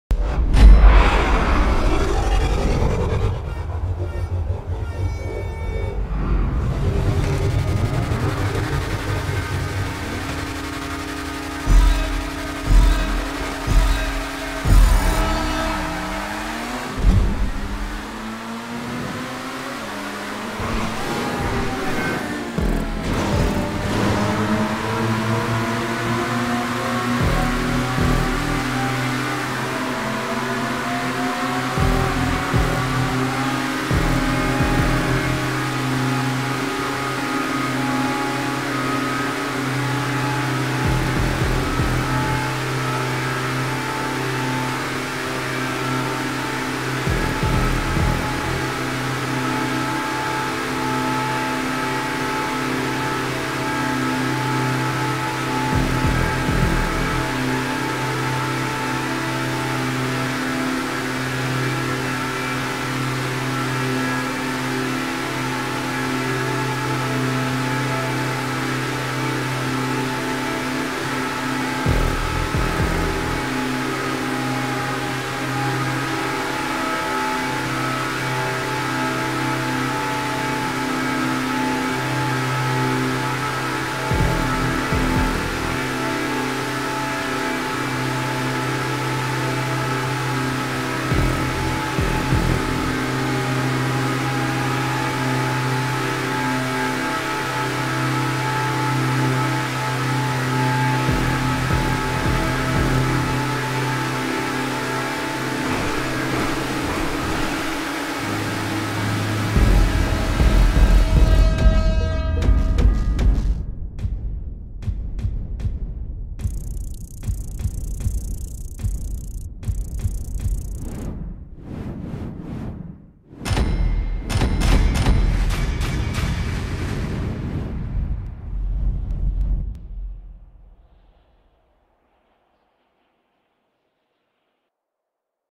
High End Wheel vs Offroad Wheel Part 2
I tested on Emerus/Krieger/D8. Result is as follows,

Result:
== Emerus ==
High End: 1:36.061
Offroad:  1:34.876
== Krieger ==
High End: 1:36.445
Offroad:  1:35.678
== Deveste Eight ==
High End: 1:23.532
Offroad: 1:20.996

Conclusion:
Dont waste of your time and money. :D